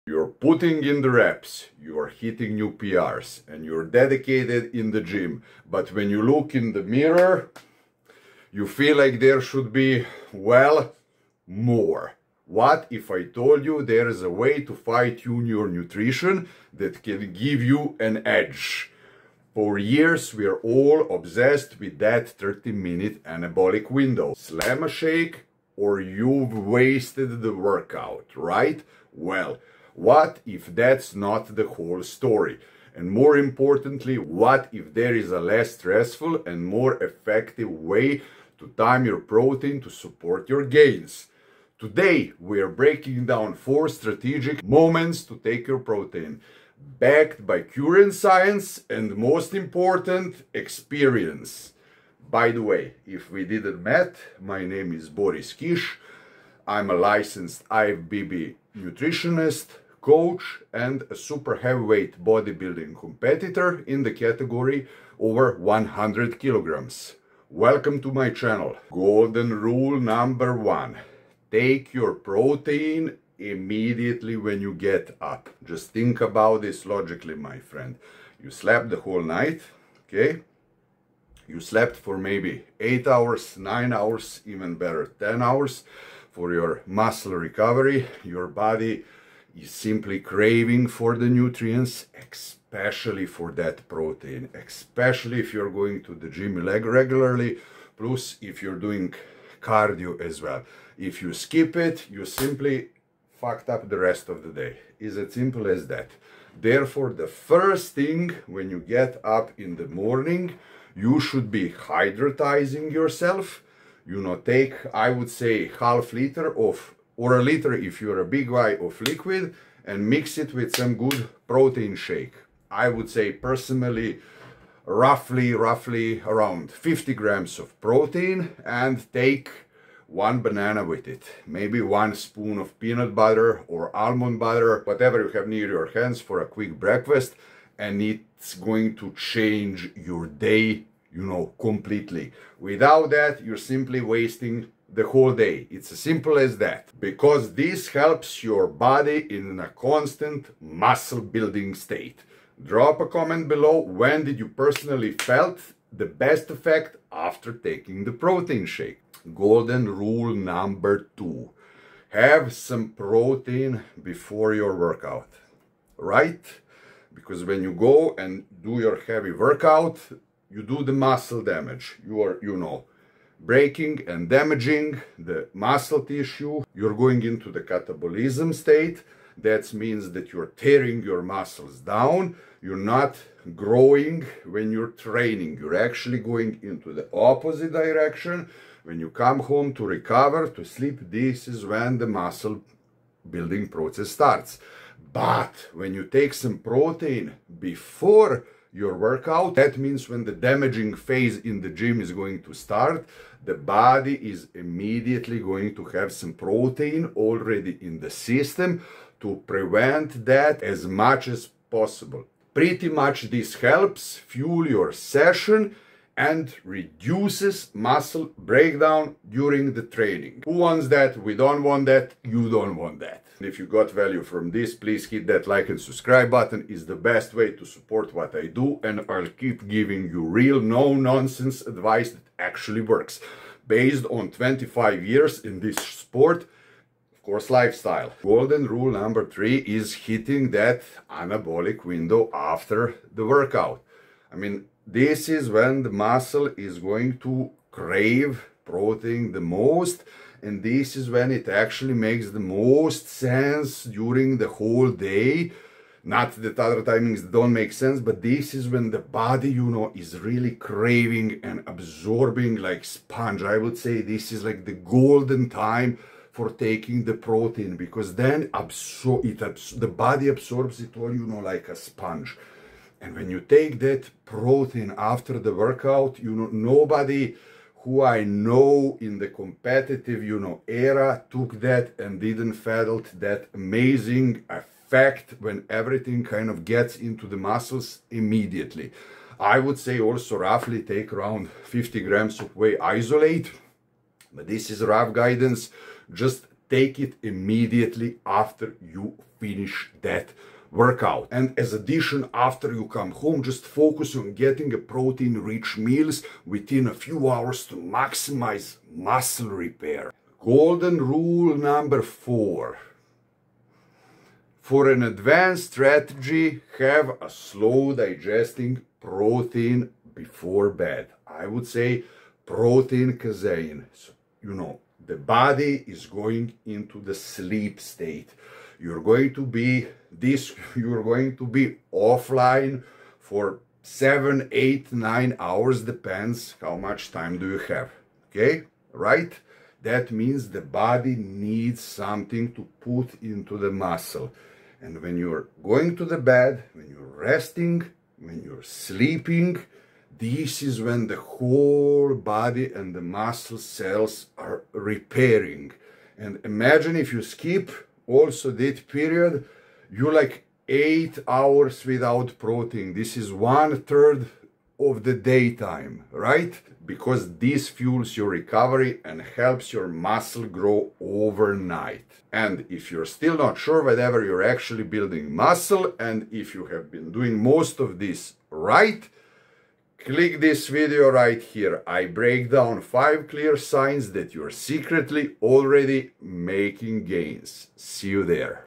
4 Golden Rules When YOU Should Take Protein
Are you training hard but not seeing the muscle growth you expect? ⏳💪
The secret might be in when you take your protein. This video debunks the myths about protein timing and gives you four science-backed golden rules 🧠✨ to optimize your nutrition for real gains.
Forget the stressful "30-minute anabolic window" ⏱️❌ and learn a more effective, flexible approach 🔄.

We’ll cover the best times to take protein ⏰ to maximize muscle protein synthesis and support your recovery 🛠️.
Learn how to use protein to your advantage by:
– spreading your intake throughout the day 🍽️
– fueling up before and after your workouts 🏋️‍♂️
– and even using a pre-sleep strategy 🌙 for overnight growth 🌱.
These practical tips ensure your body has the building blocks it needs to repair and grow 24/7 🧱🔁.

If you want to finally understand protein timing for muscle growth, build more muscle, and get the most out of every workout — this is the video for you 🎯
We cut through the noise to give you actionable advice based on current science 📚✅

Golden Rule #1: The importance of distributing protein intake evenly, starting with breakfast 🍳
Golden Rule #2: How pre-workout protein can fuel your session and reduce muscle breakdown 🏃‍♂️
Golden Rule #3: The truth about the "anabolic window" and effective post-workout nutrition 🔄💥
Golden Rule #4: An advanced strategy using slow-digesting protein before bed to enhance overnight recovery 💤🍗

Timestamps: ⌛
00:00 – Intro
01:08 – Golden Rule #1: Morning Protein – Stop Overnight Muscle Loss 🌅💪
02:39 – Golden Rule #2: Pre-Workout Protein – Fuel & Protect Your Gains ⚡🏋️
03:57 – Golden Rule #3: Post-Workout Protein – The Anabolic Window Explained ⏱️🍽️
05:11 – Golden Rule #4: Protein Before Bed – Maximize Muscle Growth Overnight 🌙📈